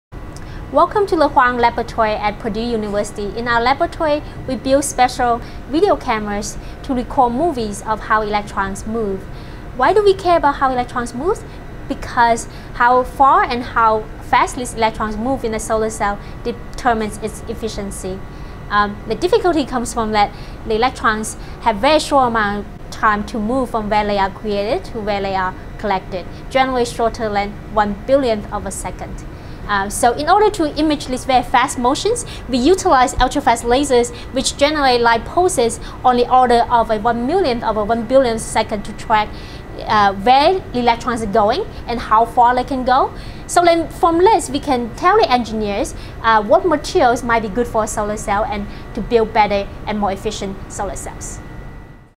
Professor Libai Huang - Efficiency of Solar Cells
Dr. Huang explains how her lab uses lasers to study organic solar cell efficiency.